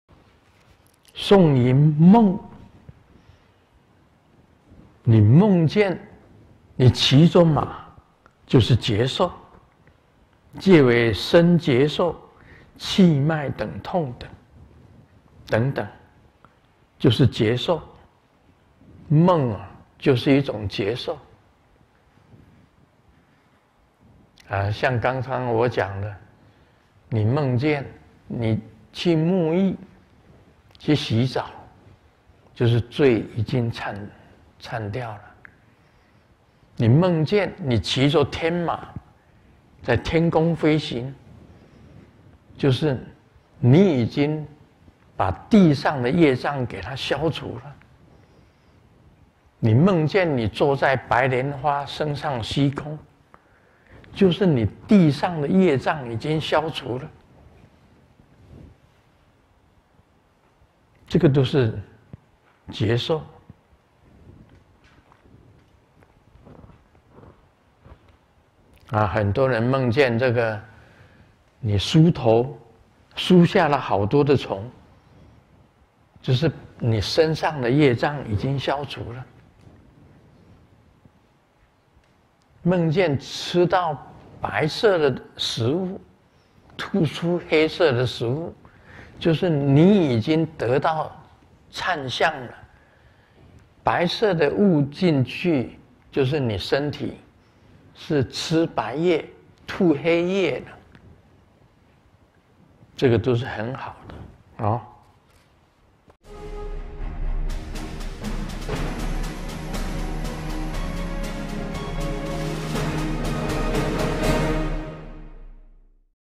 20200822聖尊蓮生活佛盧勝彥開示「道果-廣道-世間道-生定之因-總攝」：「夢」的身覺受－TBSN高清版
20200822聖尊蓮生活佛盧勝彥開示「道果-廣道-世間道-生定之因-總攝」：「夢」的身覺受－TBSN高清版